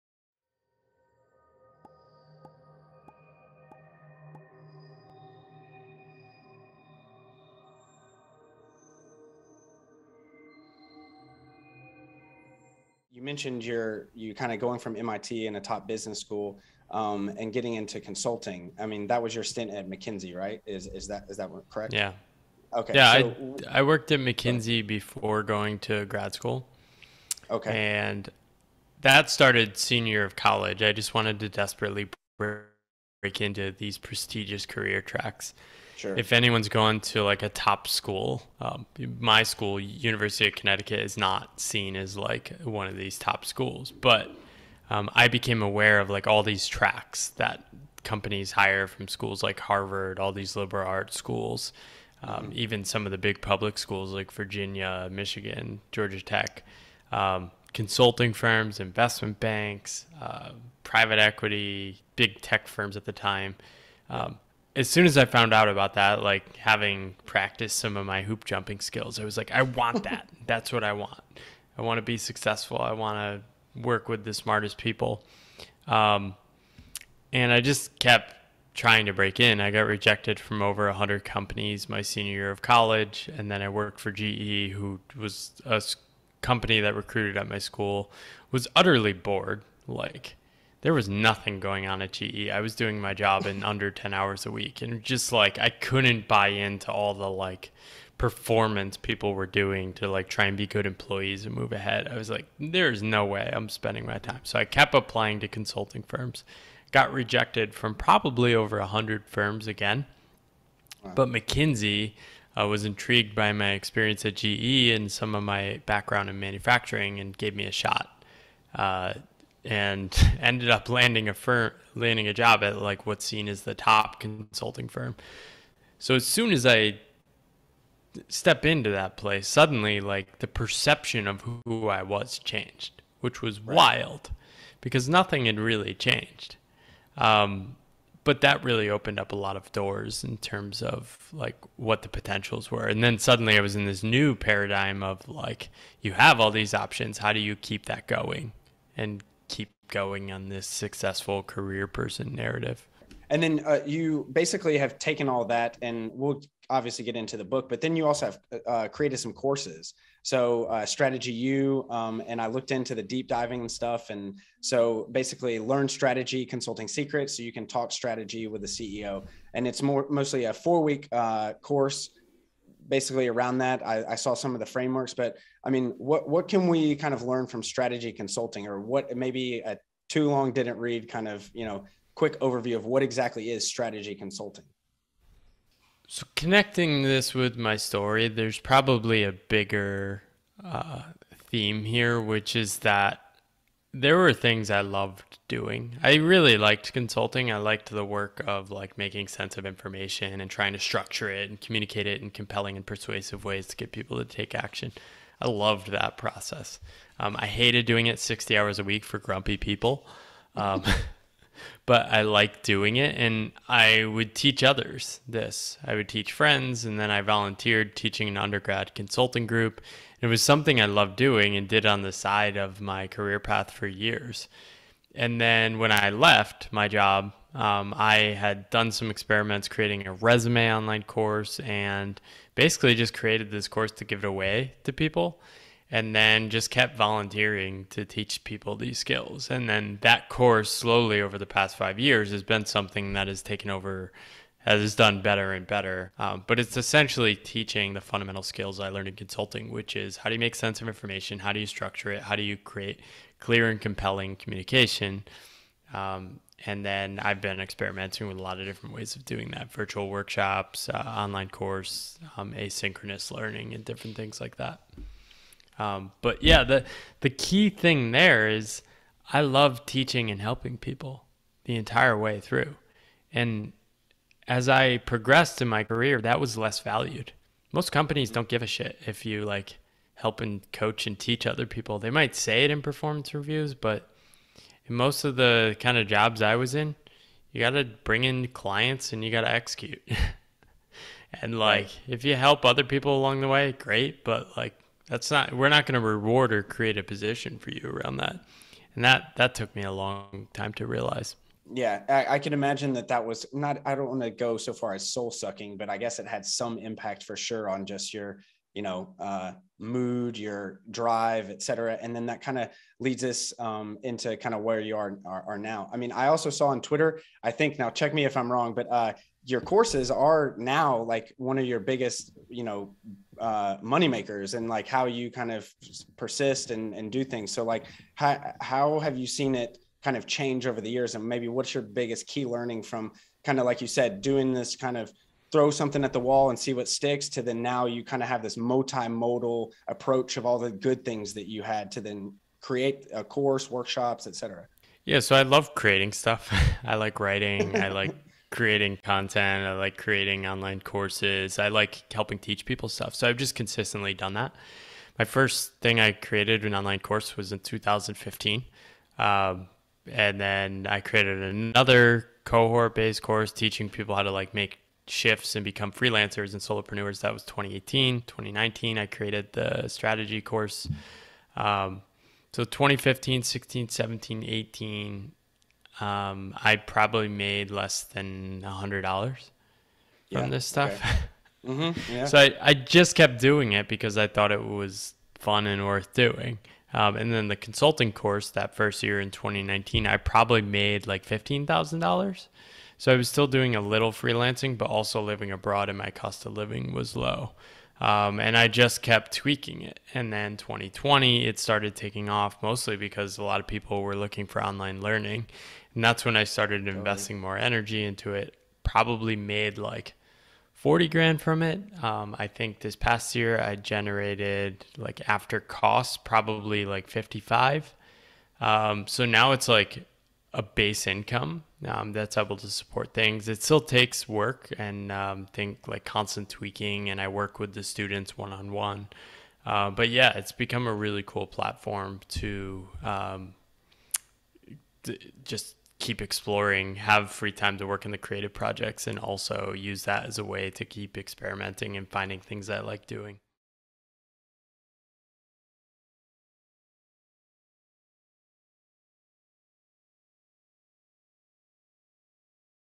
Strategy consulting and creating StrategyU | Paul Millerd | Conversations Clips
Clip from our ESconversations podcast with Writer, Coach, and Consultant, Paul Millerd